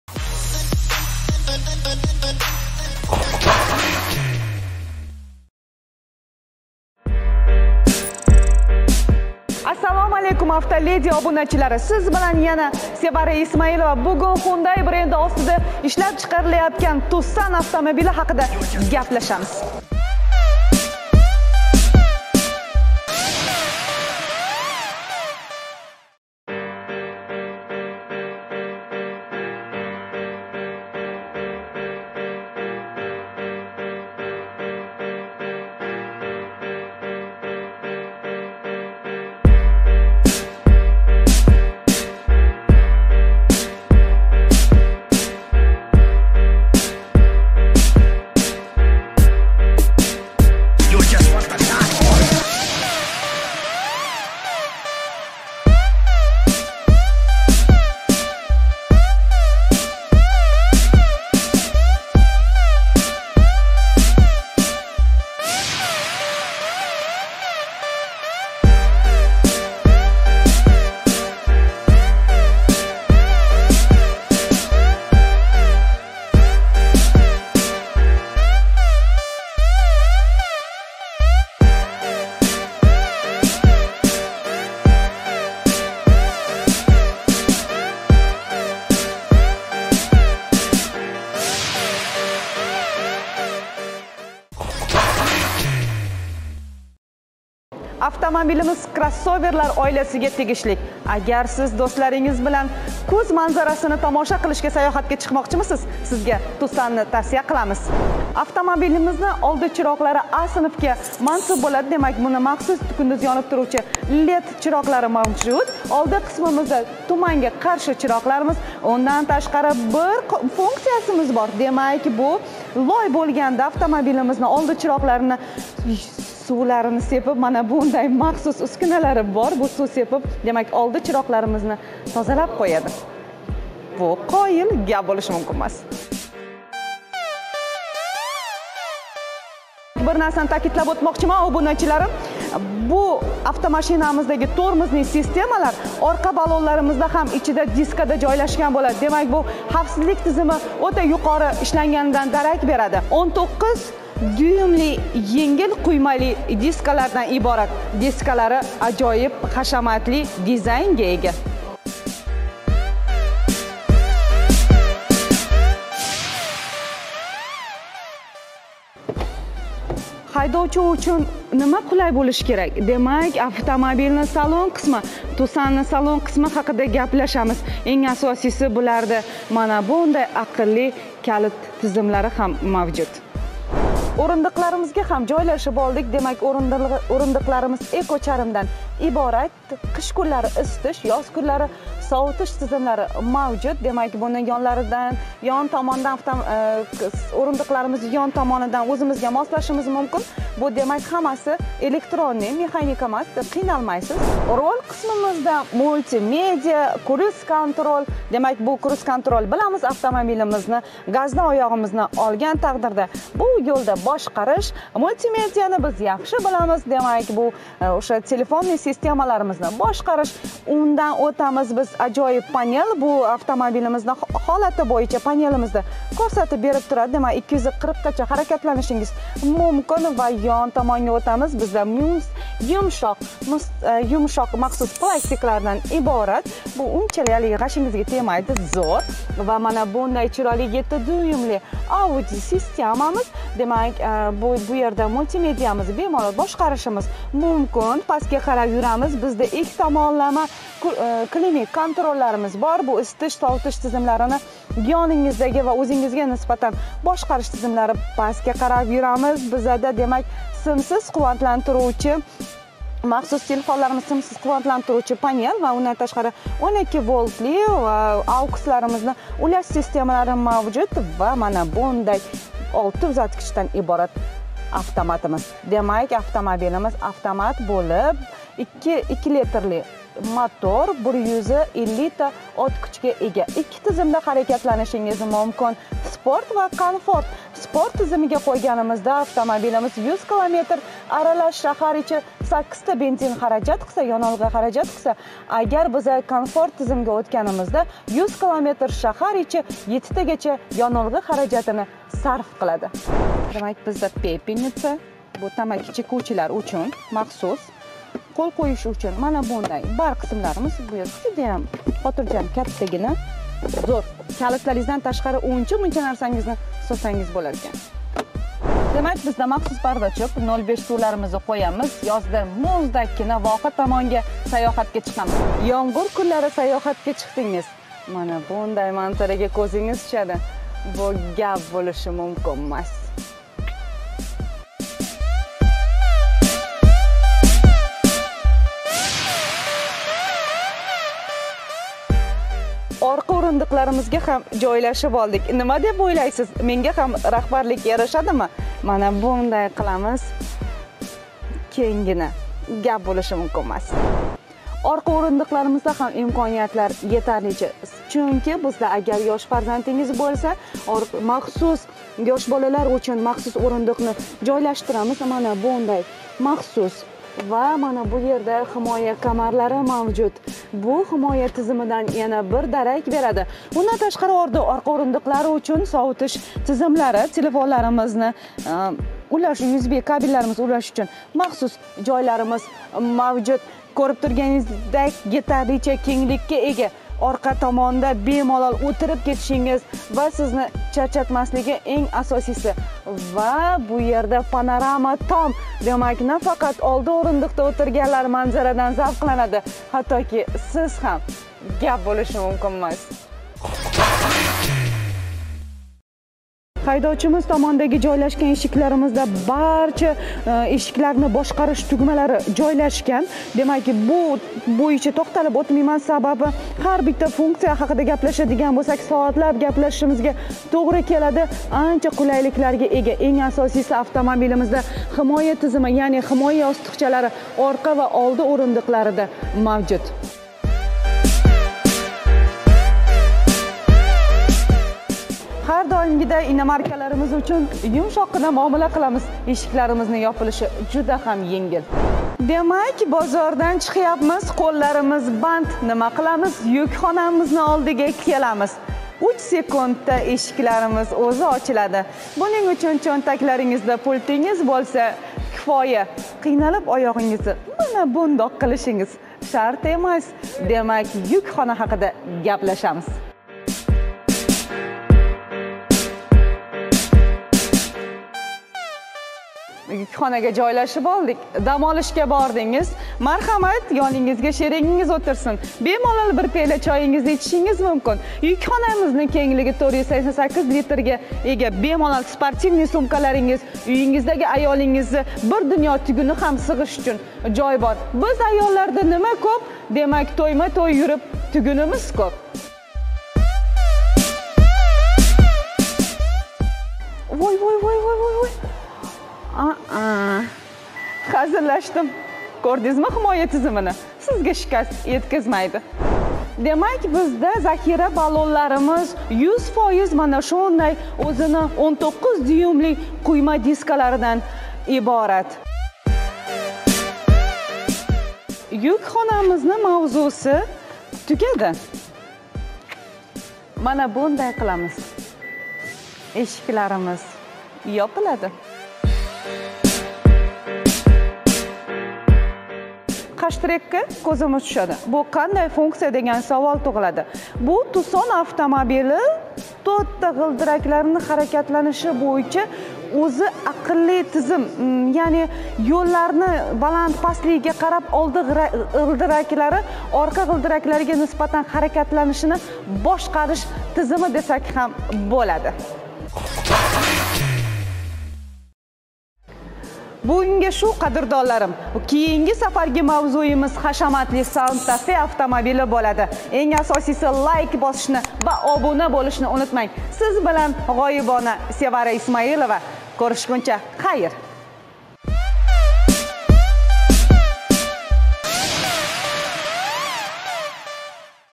endilikda 30% to`lov bilan AVTO LADY AVTO LEDI
Сизни кроссовер автомабили кизиктиряптими, у холда албатта HYUNDAI TUCSON автомабилини видеосини куришингизни тавсия киламиз.